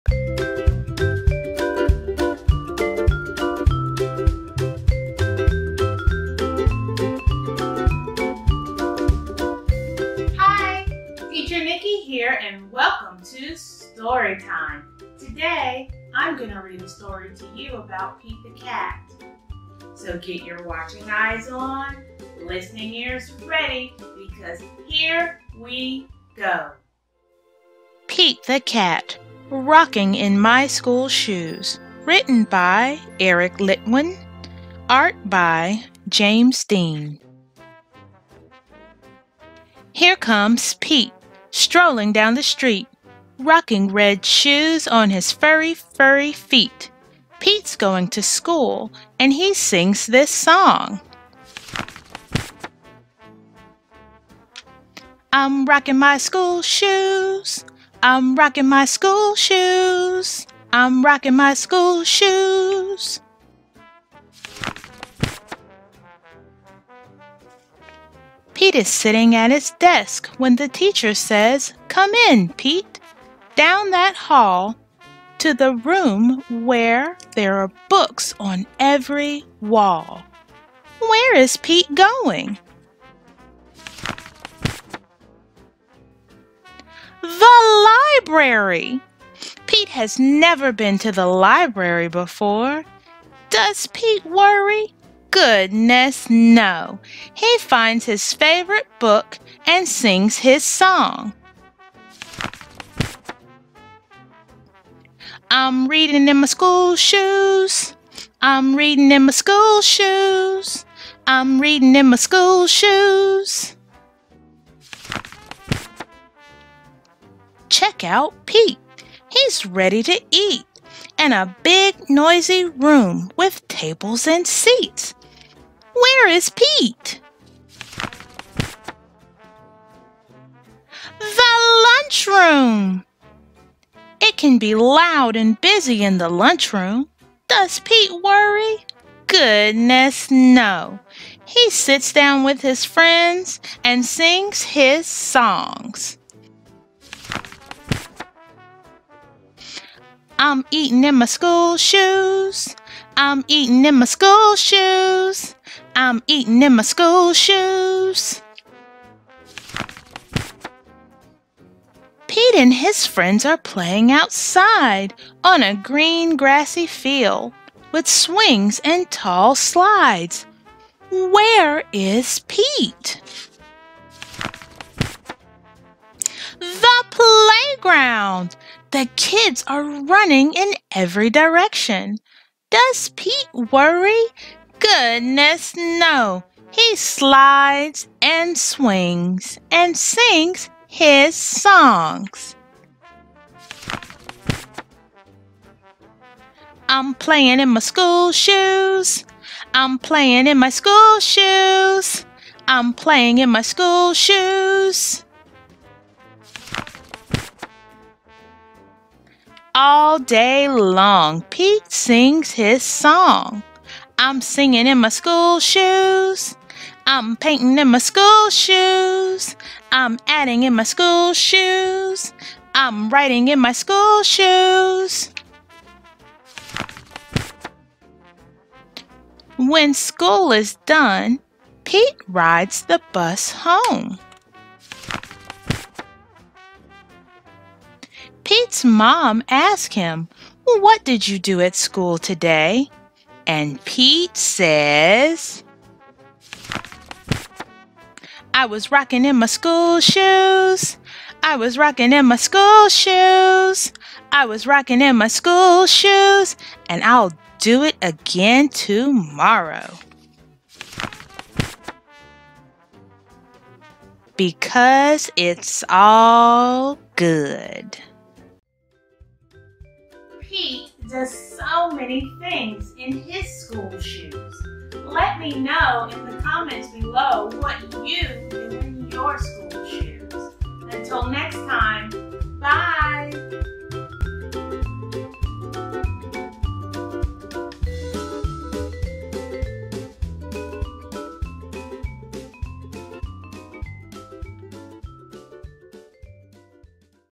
Pete the Cat Rocking in my School Shoes Children's Books Read Aloud Storytime with Teacher Nikki
Today on Storytime, Pete the Cat is rocking in his school shoes. Pete discovers the library, the lunchroom, the playground, and lots of other cool places at school. And no matter where he goes, Pete never stops moving and grooving and singing his song...because it’s all good.